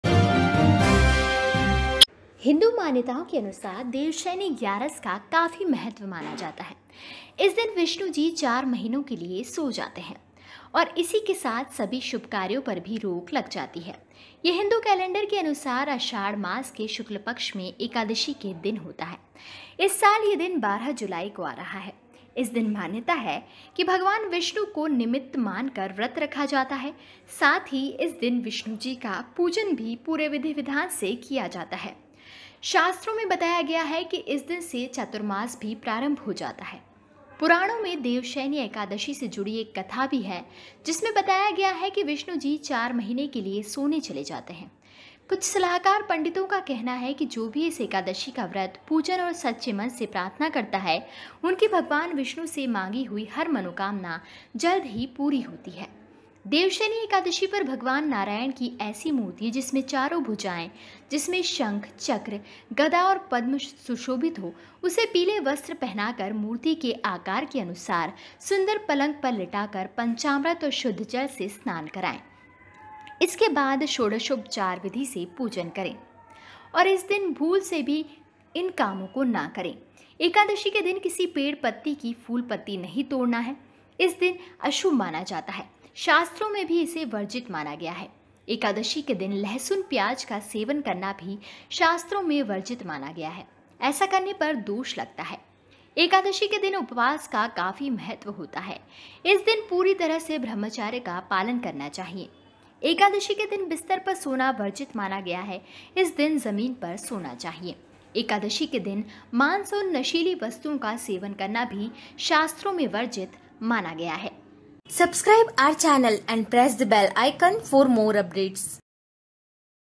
Dev Shayani Ekadashi 2019 Vrat, यह देव शयनी एकादशी बदल सकती है आपका भाग्य
जानिए इस दिन  क्या करना होगा | Dev Shayani Ekadashi 2019 Vrat

हिन्दू मान्यताओं के अनुसार देवशयनी ग्यारस का काफी महत्व हैं माना जाता हैं कि इस दिन विष्णु जी 4 महीनों के लिए सो जाते हैं और इसी के साथ सभी शुभ कार्यों पर भी रोक लग जाती है। यह हिन्दू कैलेंडर के अनुसार आषाढ़ मास के शुक्ल पक्ष में एकादशी के दिन आता है। इस साल यह दिन 12 जुलाई को आ रहा हैं। इस दिन मान्यता हैं कि भगवान विष्णु को निमित्त मान कर व्रत रखा जाता हैं और साथ ही इस दिन विष्णु जी का पूजन भी पूरे विधि विधान से किया जाता हैं ।

शास्त्रों में बताया गया हैं कि इस दिन से चातुर्मास भी प्रारंभ हो जाता है। पुराणों में देवशयनी एकादशी से जुड़ी एक कथा भी हैं जिसमे बताया गया है कि विष्णु जी चार महीने के लिए सोने चले जाते हैं। कुछ सलाहकार पंडितों का कहना कि जो भी इस एकादशी का व्रत, पूजन और सच्चे मन से प्रार्थना करता है, उनकी भगवान विष्णु से मांगी हुई हर मनोकामना जल्द ही पूरी होती है।